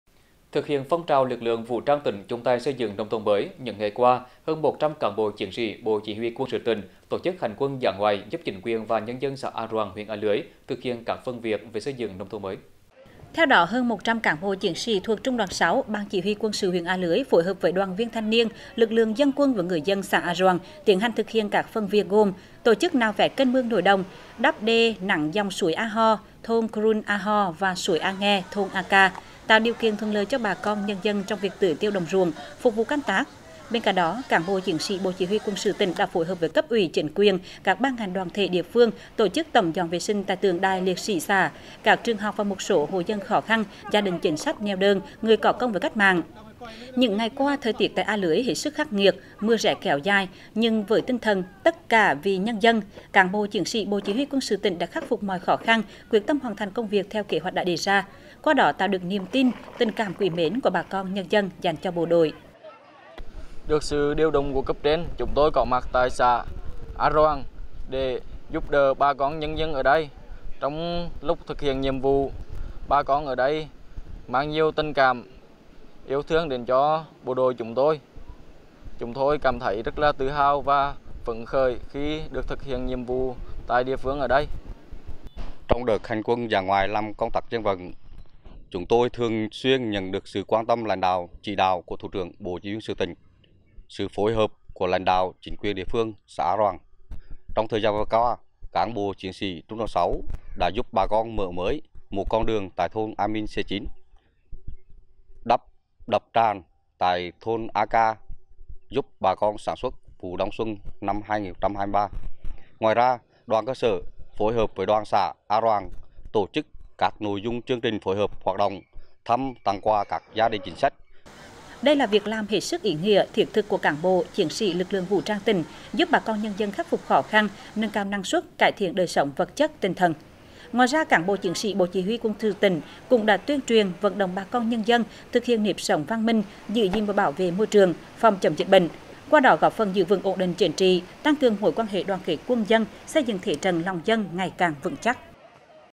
Bộ CHQS tỉnh hành quân dã ngoại làm công tác Dân vận tại huyện A Lưới
Thực hiện phong trào “Lực lượng vũ trang tỉnh chung tay xây dựng nông thôn mới”, những ngày qua, hơn 100 cán bộ, chiến sĩ Bộ Chỉ huy Quân sự tỉnh tổ chức hành quân dã ngoại giúp chính quyền và nhân dân xã A Roàng, huyện A Lưới thực hiện các phần việc về xây dựng nông thôn mới.